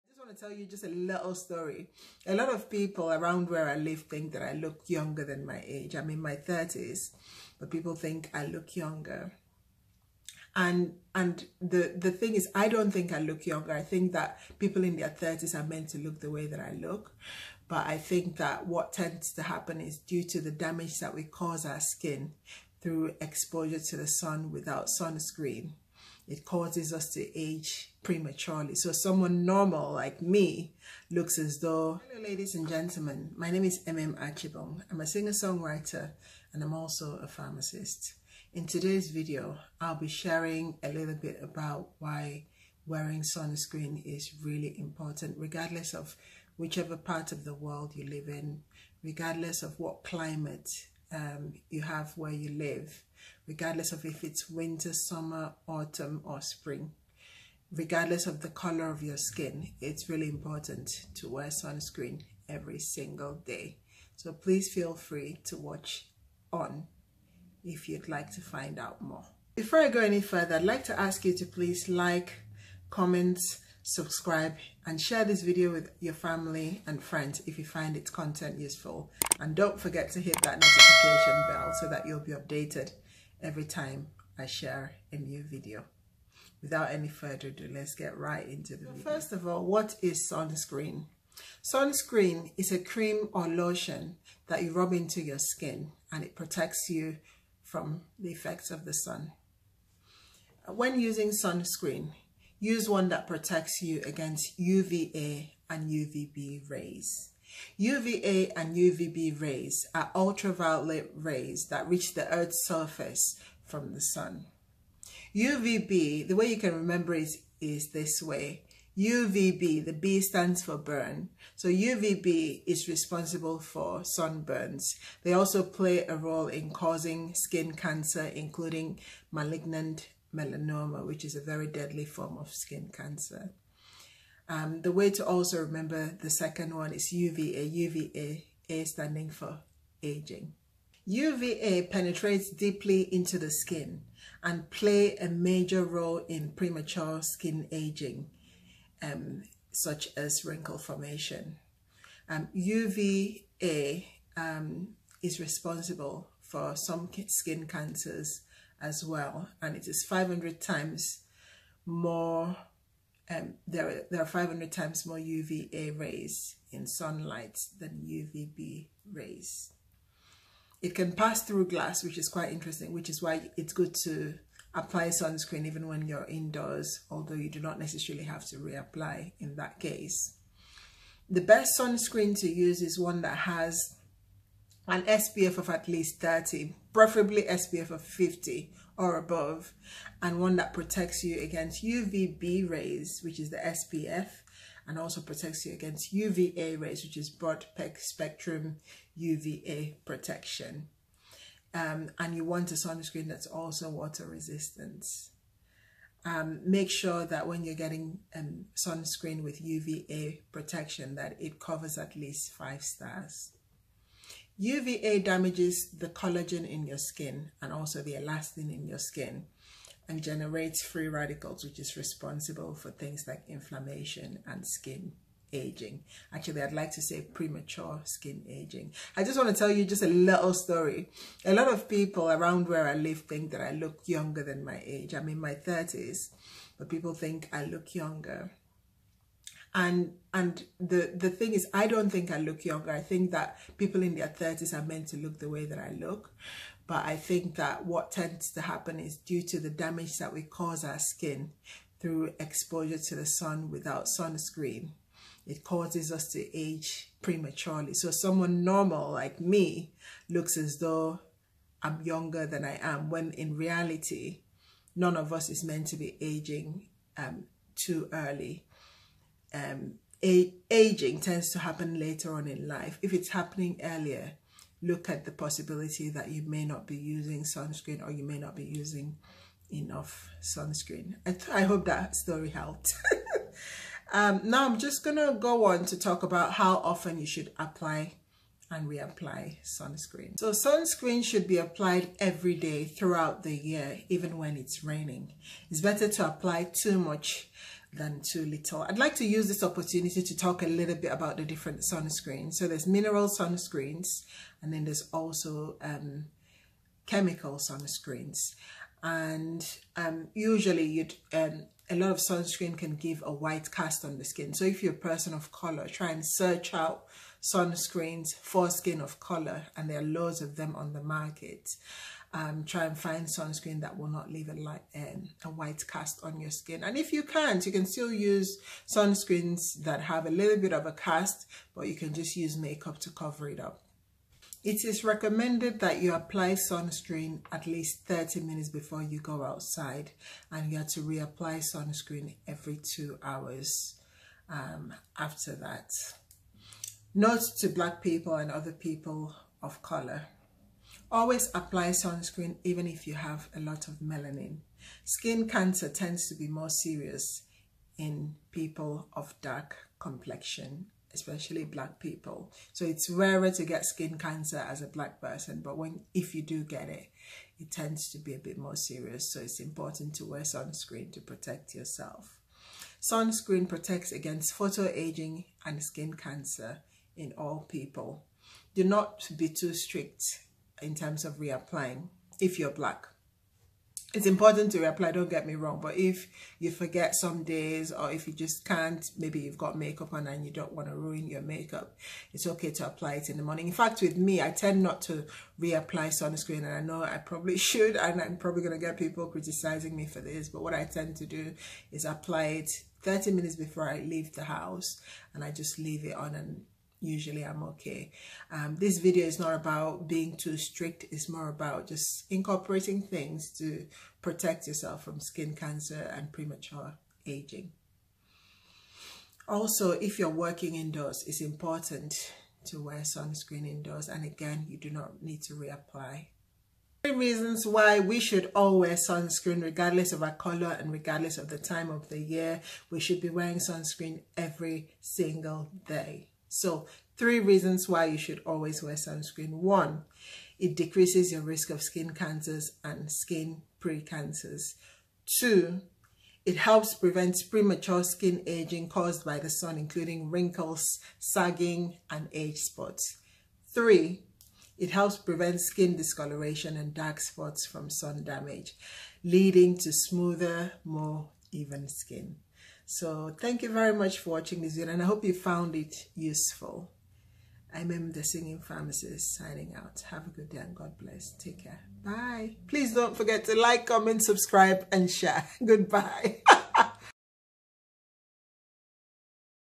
Why You Should Wear Sunscreen Everyday Even When It's Winter Or You Have Dark Skin By Emem Archibong
What Is Sunscreen?
It is a cream or lotion rubbed onto the skin to protect it from the sun.

When using a sunscreen, use one that protects you from UVA and UVB rays.

UVA and UVB rays are ultraviolet rays that reach the earth's surface from the sun.

UVB - B - BUrn (responsible for sunburns). They also play a role in causing skin cancer including malignant melanoma.

UVA - A - Aging (also play a role in skin cancer)

Penetrates deeply into the skin and play a major role in premature skin aging changes including wrinkle formation (photoaging)

500 times more UVA rays in sunlight than UVB rays.

Can pass through glass

SPF 50 and broad spectrum UVA and water resistant

UVA cover should be at least 5 stars

UVA damages the collagen and elastin in the skin and also generates free radicals.

How often should you apply and reapply

SPF - UVB

It should be applied everyday through out the year even when it's raining. Better to apply too much than too little.

It is recommended that it is applied at least 30 minutes before going out and reapplied every 2 hours

Note to black people and other dark skinned people

Always apply sunscreen even if you have a lot of melanin. Skin cancer tends to be more serious in black people

Sunscreen protects against photoaging and skin cancer in all people

Do not be too strict about reapplying but make sure you always apply enough.
Also if working in doors, you may not need to reapply.

Why you should apply sunscreen?

1. Decreases your risk of skin cancers and skin pre cancers

2. Helps prevent premature skin aging caused by the sun including wrinkles, sagging and age spots

3. It helps prevent skin discolouration and dark spots from sun damage leading to smoother more even skin.

Facebook.com/emem.archibong
Instagram.com/ememarchibongofficial
Twitter.com/ememmusic